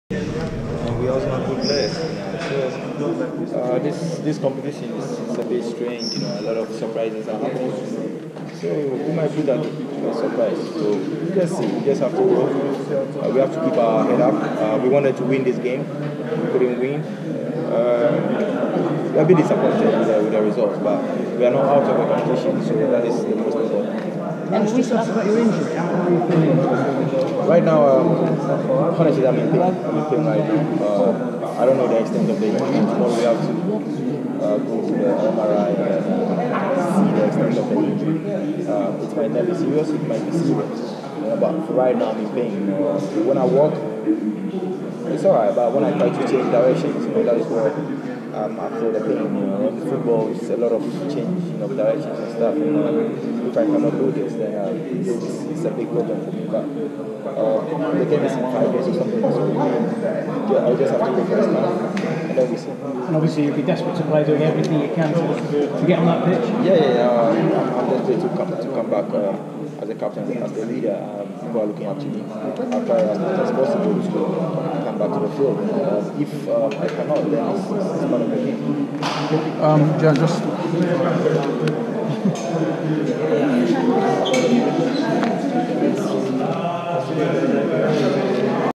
Asamoah Gyan talks about 'very strange' Afcon 2017
Ghana striker speaks after 0-1 loss to Egypt in final group game and also bemoans his injury